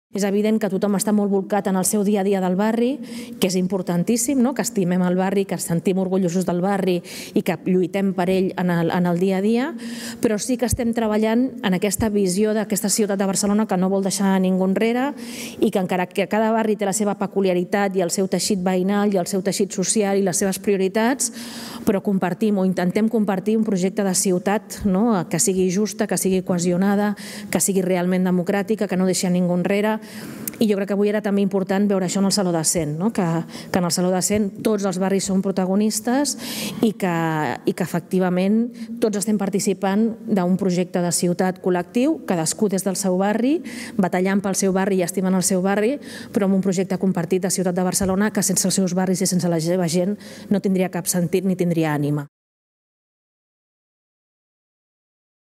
Trobada amb els grups impulsors del Pla de barris
Els grups impulsors dels 16 barris són una peça clau del Pla de barris per posar en marxa més de 350 accions per reduir les desigualtats en els barris que més ho necessiten.